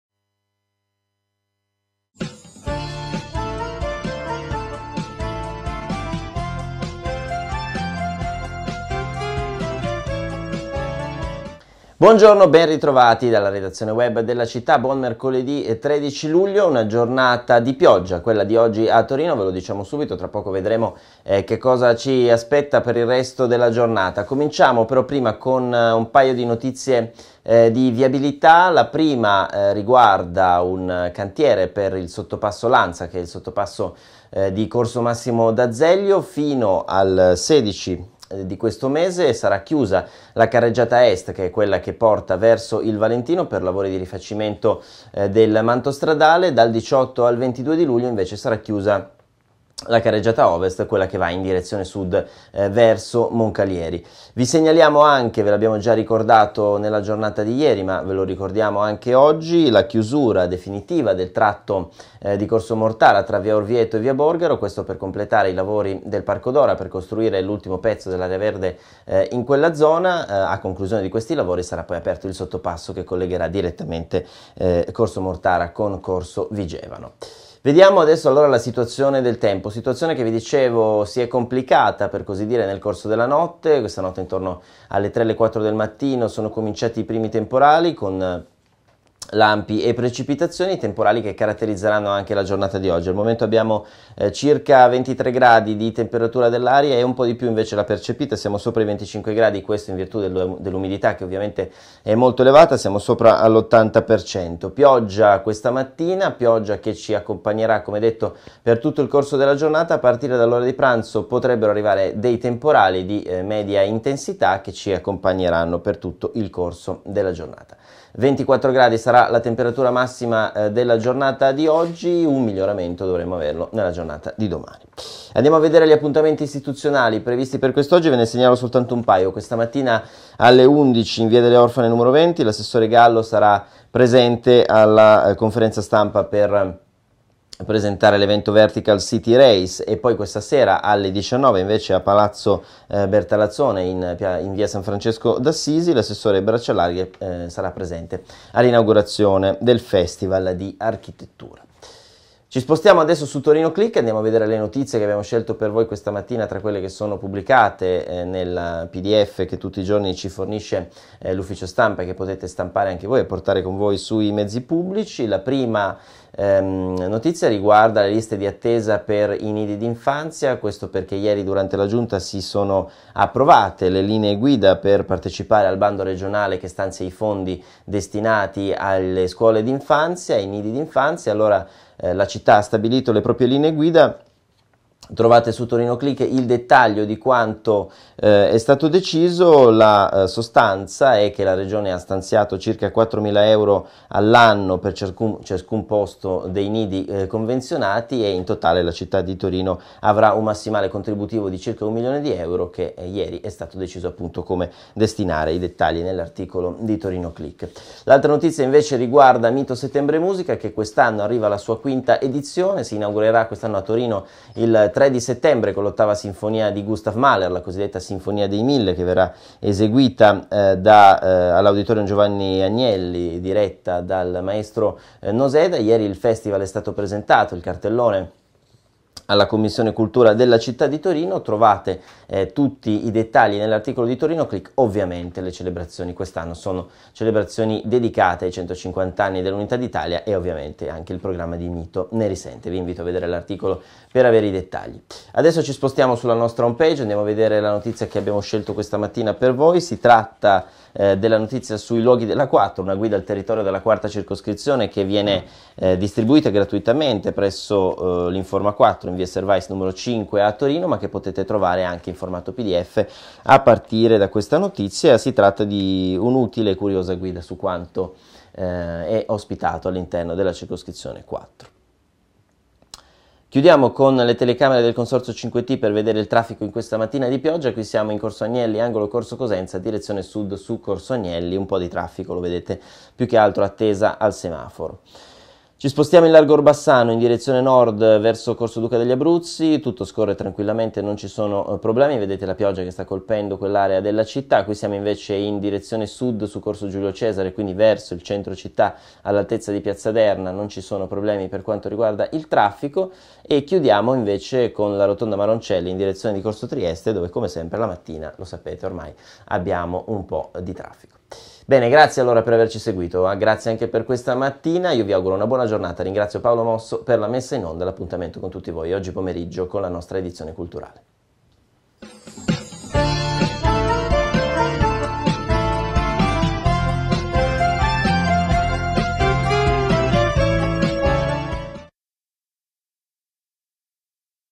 TWN quot. n. 1414 di mercoledì 13/07/2011 - ore 09.15
Giornata di pioggia a torino con possibili temporali a partire dall'ora di pranzo. Da TorinoClick: attese più brevi per i nidi d'infanzia; presentazione di MiTo Settembre Musica 2011.